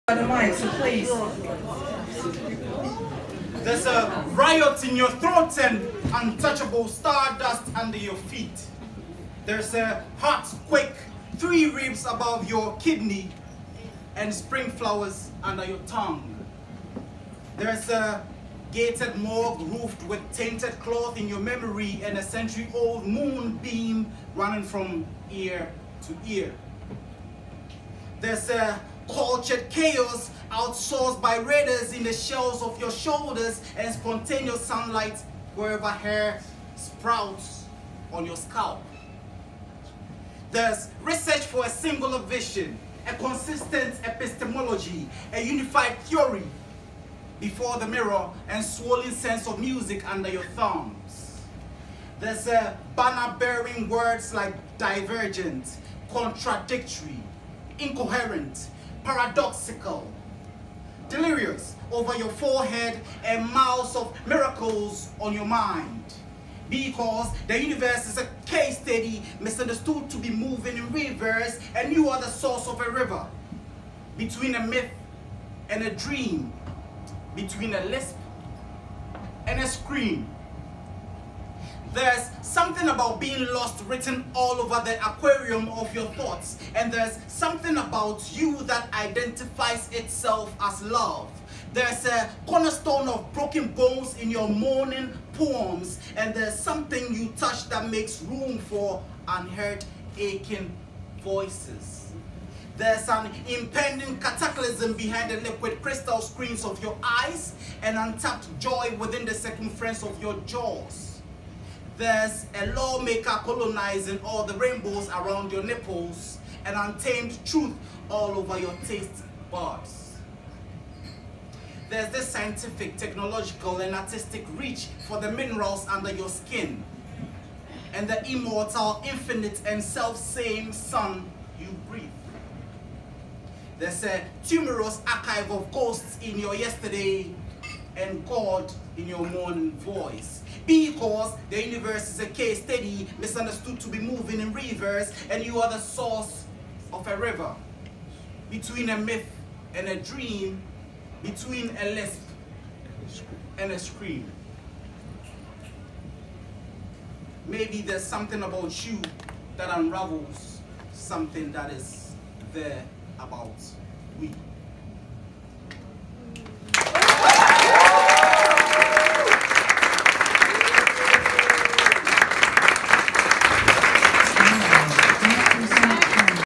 Kwame Write at SASAA open mic, Copenhagen
LYRICS:

There’s a riot in your throat and untouchable stardust under your feet
There’s a heartquake 3 ribs above your kidney and spring flowers under your tongue
There’s a gated morgue roofed with tainted cloth in your memory and a century old moon beam running from ear to ear
There’s a cultured chaos outsourced by raiders in the shelves of your shoulders and spontaneous sunlight wherever hair sprouts on your scalp
There’s research for a singular vision, a consistent epistemology, a unified theory before the mirror and swollen sense of music under your thumbs
There’s a banner bearing words like divergent, contradictory, incoherent, paradoxical, delirious over your forehead and miles of miracles on your mind

Because the universe is a case study misunderstood to be moving in reverse and you’re the source of a river
Between a myth and a dream
Between a lisp and a scream

There’s something about being lost written all over the aquarium of your thoughts and there’s something about you that identifies itself as love
There’s a cornerstone of broken bones in your moaning poems and there’s something you touch that makes room for unheard aching voices
There’s an impending cataclysm behind the liquid crystal screens of your eyes and untapped joy within the circumference of your jaws
There’s a lawmaker colonizing all the rainbows around your nipples and untamed truth all over your tastebuds
There’s this scientific, technological, and artistic reach for the minerals under your skin and the immortal, infinite, and selfsame sun you breathe
There’s a tumorous archive of ghosts in your yesterday and God in your morning voice

Maybe there’s something about you that unravels something that is there about we

(wrote and performed on 30.03.19 at SASAA, Copenhagen)